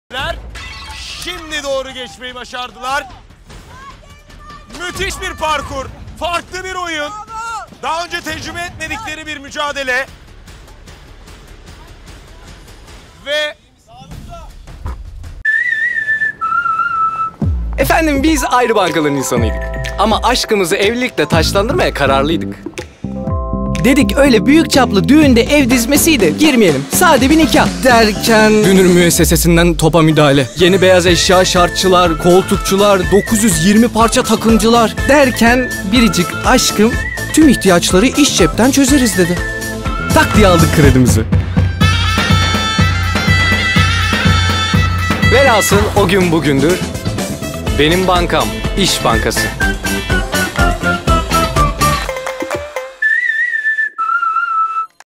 İş Bankası | TV8 | Altbant
TVEkstra'nın İş Bankası için TV8'e hazırlamış olduğu reklam önizlemesi